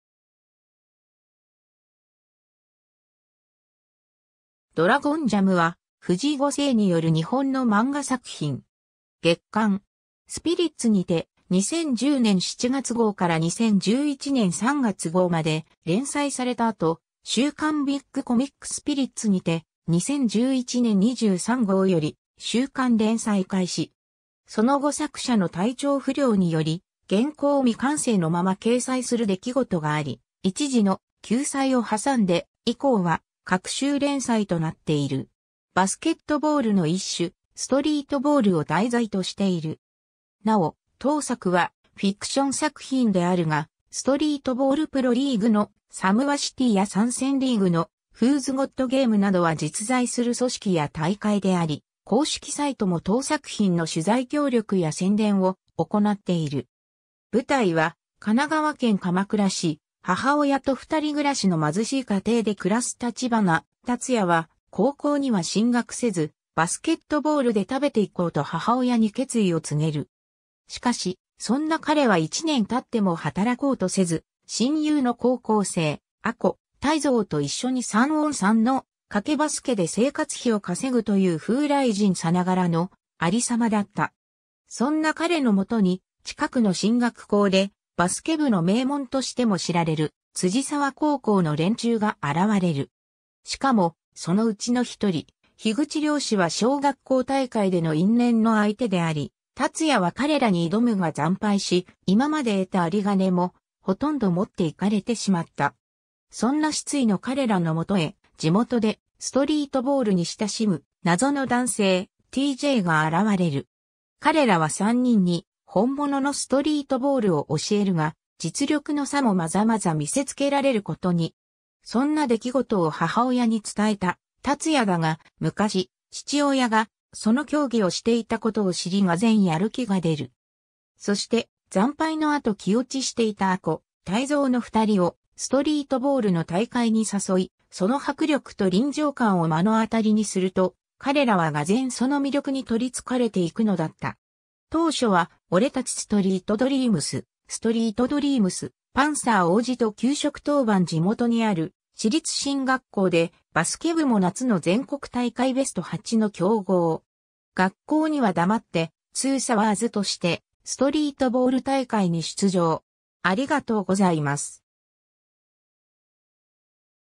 DRAGON JAM
月刊!スピリッツ
『DRAGON JAM』（ドラゴンジャム）は、藤井五成による日本の漫画作品。
『月刊!スピリッツ』（小学館）にて、2010年7月号から2011年3月号まで連載された後、『週刊ビッグコミックスピリッツ』（同社刊）にて、2011年23号より週刊連載開始。
その後作者の体調不良により原稿を未完成のまま掲載する出来事があり、一時の休載を挟んで以降は隔週連載となっている。
バスケットボールの一種、ストリートボール（一般にはストリートバスケと呼ばれることが多いが、本編では正式名称のストリートボールで統一しており、本記事もストリートボールで記述する）を題材としている。
 尚、当作はフィクション作品であるが、ストリートボールプロリーグの「SOMECITY」や参戦リーグの「WHO'S GOT GAME」などは実在する組織や大会であり、公式サイトも当作品の取材協力や宣伝を行っている。
 舞台は神奈川県鎌倉市、母親と二人暮らしの貧しい家庭で暮らす立花龍也は、高校には進学せず、バスケットボールで食べていこうと母親に決意を告げる。
しかし、そんな彼は1年経っても働こうとせず、親友の高校生アコ、タイゾーと一緒に3on3の賭けバスケで生活費を稼ぐという風来人さながらの有様だった。
そんな彼の元に近くの進学校でバスケ部の名門としても知られる辻沢高校の連中が現れる。
しかも、そのうちの一人、緋口涼司は小学校大会での因縁の相手であり、龍也は彼らに挑むが惨敗し、今まで得た有り金もほとんど持って行かれてしまった。
そんな失意の彼らの元へ地元でストリートボールに親しむ謎の男性、TJ（ティージェイ）が現れる。
彼らは三人に本物のストリートボールを教えるが、実力の差もまざまざ見せ付けられることに。
そんな出来事を母親に伝えた龍也だが、昔父親がその競技をしていたことを知り俄然やる気が出る。
そして、惨敗の後気落ちしていたアコ、タイゾーの二人をストリートボールの大会に誘い、その迫力と臨場感を目の当たりにすると、彼らは俄然その魅力に取り憑かれていくのだった。
 当初は「俺達ストリートドリームス」→ 「ストリートドリームス」→「パンサー王子と給食当番」 地元にある私立進学校で、バスケ部も夏の全国大会ベスト8の強豪。
学校には黙って「TWO-サワーズ」としてストリートボール大会に出場。